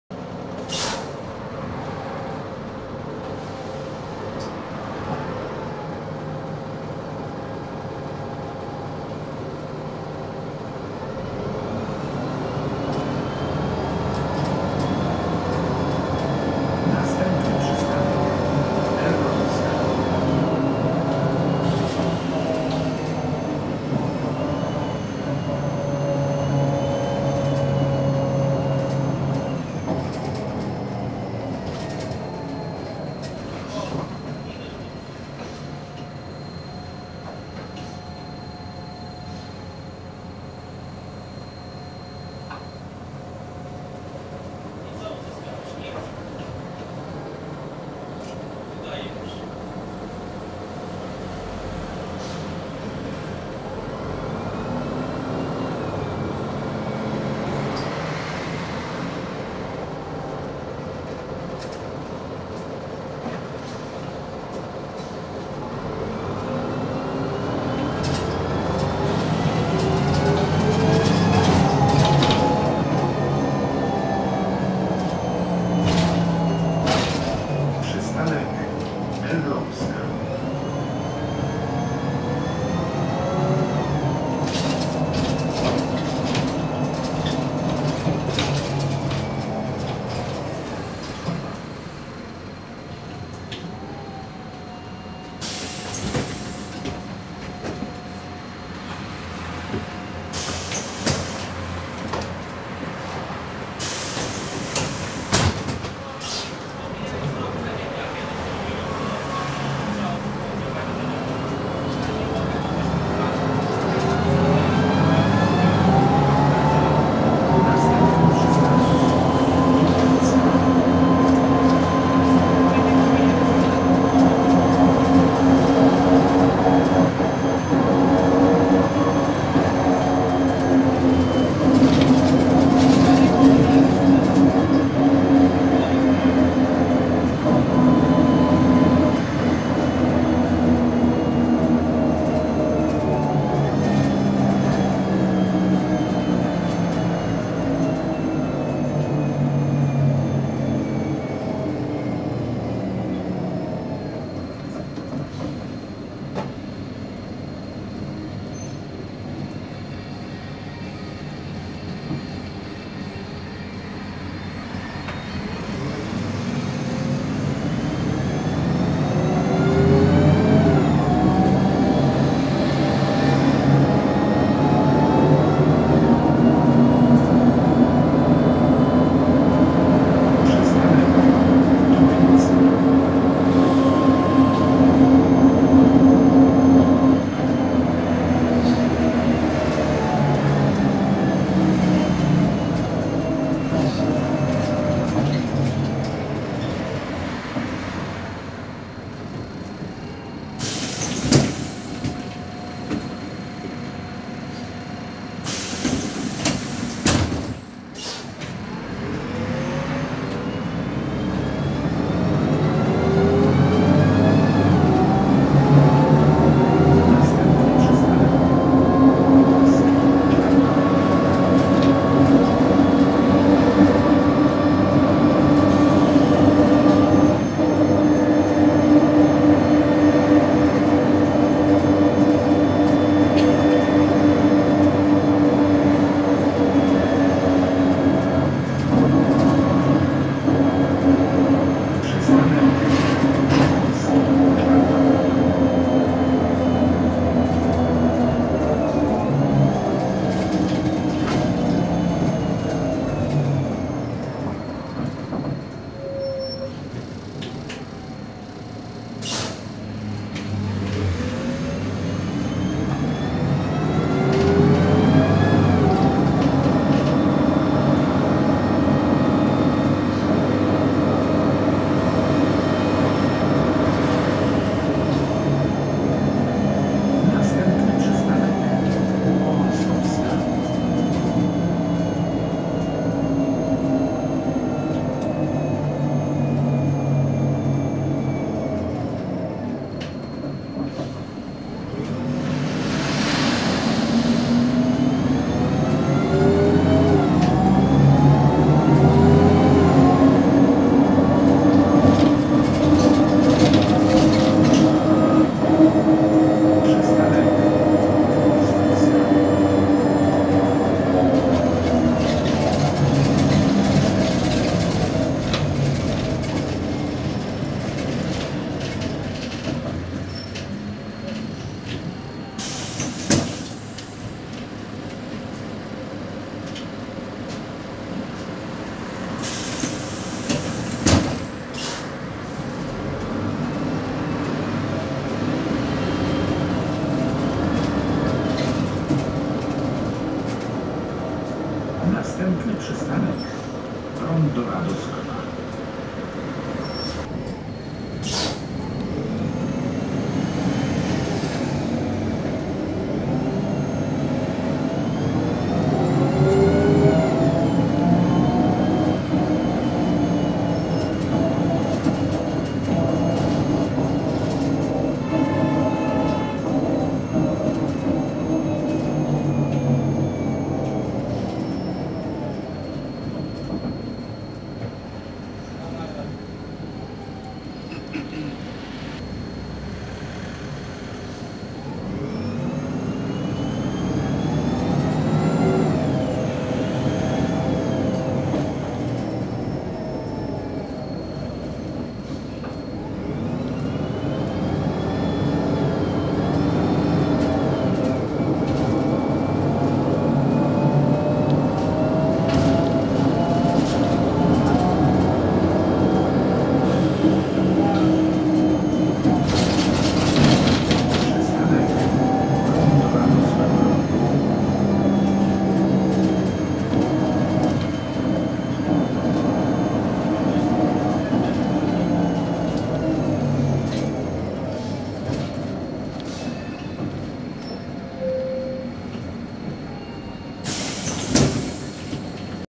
Jelcz M121I4 #4922 [🚍➡️ 327 - MZA Warszawa - Przejazd]
DANE TECHNICZNE
Autobus - Jelcz M121I4 #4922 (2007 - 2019) [*]
Rocznik - 2007
Silnik - Iveco F4AE3682E (264KM)

INFORMACJE
Linia i kierunek - 327 [Powązki - Cm. Wojskowy - C.H. Targówek] (Linia zwykła - okresowa)